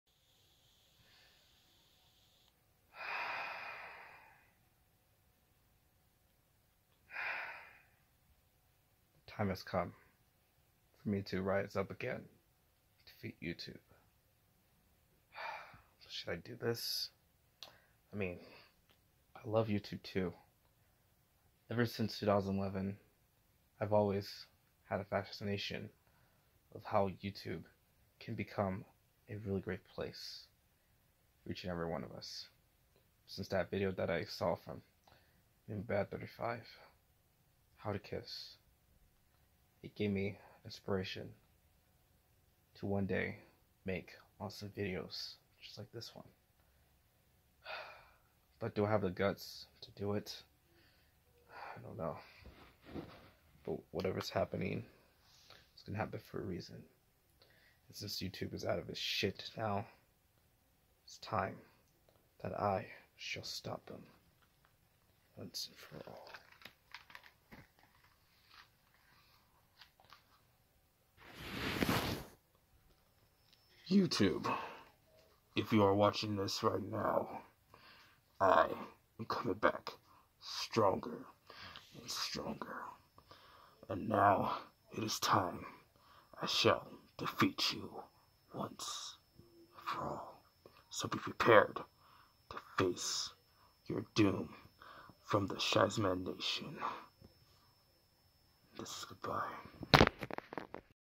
A secret message
After Suffering a defeat by the hands of YouTube. Shysman 21 is back and has a message for YouTube